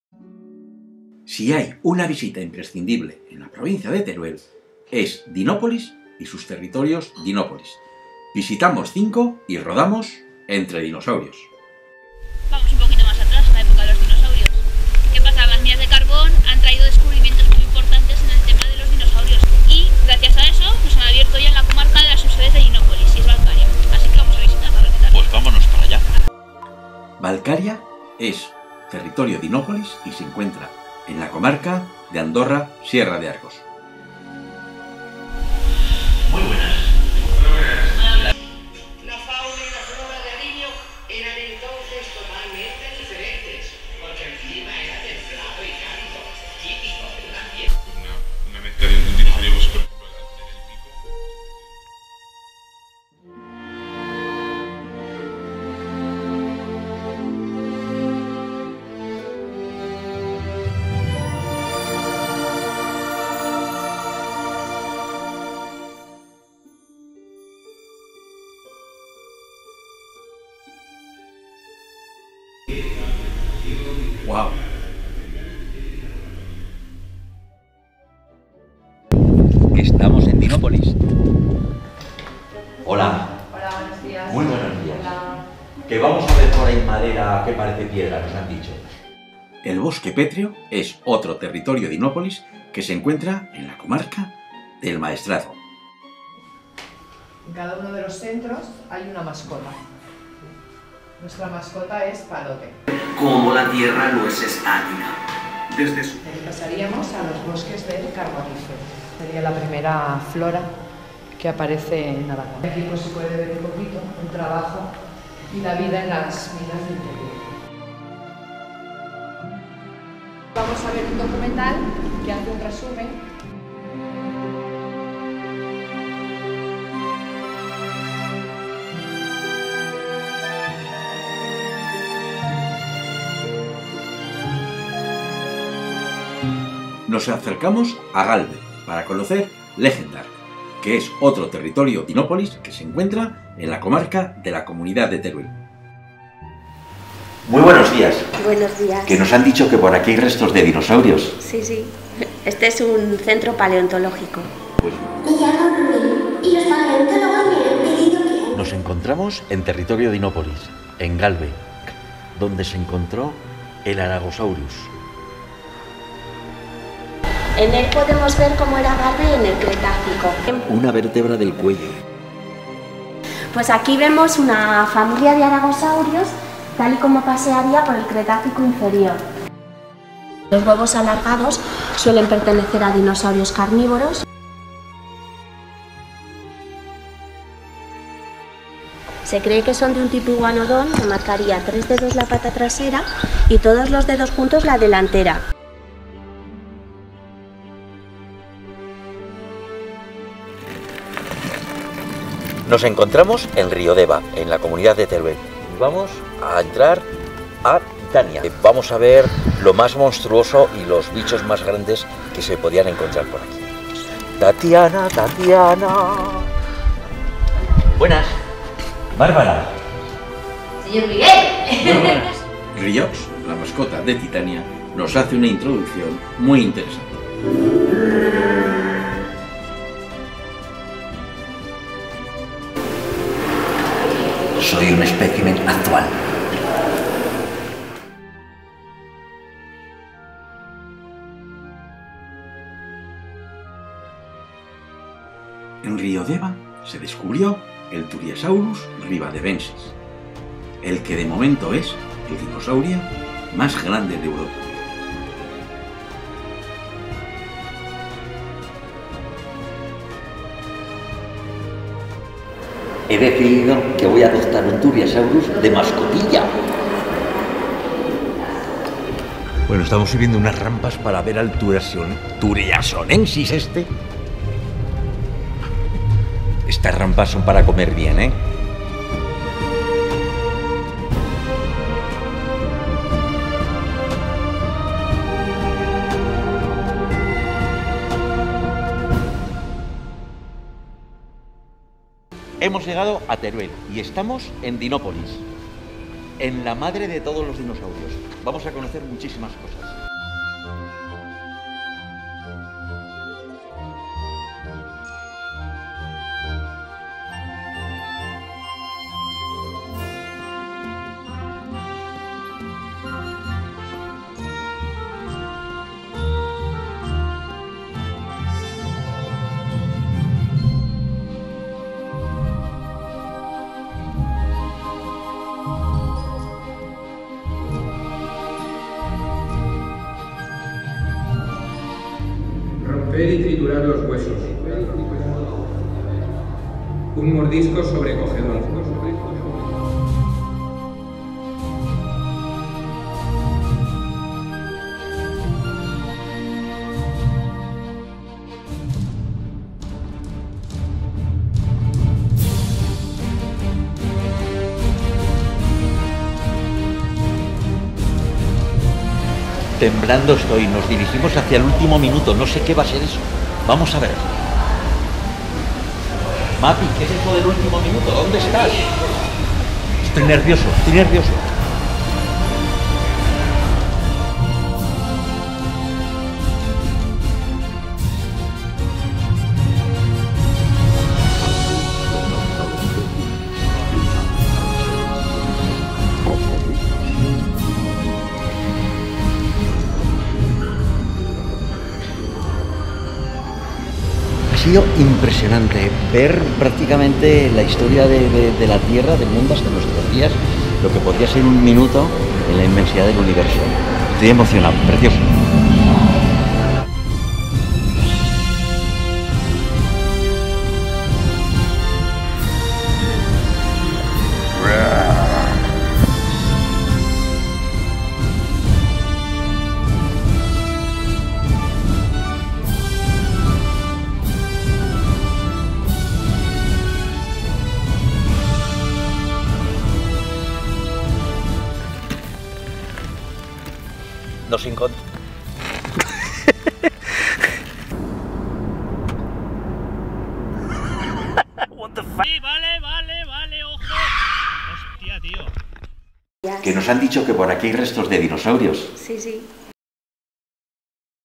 Territory Dinópolis in Teruel with wheelchair
In our tour of the most beautiful and accessible places wheelchair by the Province of Teruel and you can see by clicking here, and here, visited the headquarters of Dinópolis and 4 of the 7 satellite sub-offices throughout the province.

As you can see in the video of my personal experience discovering the world of dinosaurs, we visited Valcaria in Ariño, region of Andorra-Sierra de Arcos and where we will learn the different deposits and fossils found in the coal mine and flora and fauna of the Cretaceous. Access is wheelchair accessible and has disabled toilets.

The following is based Pétreo visit Forest in Castellote, Maestrazgo region, where we will discover the first plant of Aragon found, as well as plant discoveries found in the coal mine. There is ample parking, but without spaces reserved for the disabled, accessed without architectural barriers and also has adapted bathrooms.

We go to the region of Comunidad de Teruel, where we visit two subsites: Legendark in Galve focused on the discovery Aragosaurus and life in the Early Cretaceous, has an outside area with tables and a life-size reproduction of a we can see traces roadside, although Legendark we ensure accessibility. It is also wheelchair accessible, it has adapted bathrooms and ample parking space but without signaling any larger. The other sub-site we visited is Titania, in Riodeva. dealing gigantism also know the largest dinosaur in Europe and was found in this locality, the Turiasaurus Rivadevensis. There is parking area unmarked, accessed without architectural barriers and also has adapted bathrooms.

We leave for the end to "The mother of all dinosaurs," Dinopolis, headquarters, located in the city of Teruel in the region Community of Teruel. It is spectacular, from the Paleontological Museum, to the world of T.Rex, through shows and attractions so amazing and instructive as the "last minute" that will have already seen in the video and if not, not to hope. We can spend a whole day quietly enjoying himself. Has several places of special and wider parking, wheelchair access and of course has adapted bathrooms.

In the province of Teruel have made significant paleontological discoveries and a reference place to share with all the traveling and travelers regardless of disability, Territory Dinópolis with its headquarters and 7 satellite offices throughout the province of Teruel with the discoveries made in each of these areas.

Video shown 4 satellite offices and the central and as you can see enjoyed as a child, I have trained well and I personally I can recommend the.

Thanks to Dinópolis for their cooperation, to the province of Teruel and all for joining us on this trip.
Traductor de Google para empresas:Google Translator ToolkitTraductor de sitios webGlobal Market Finder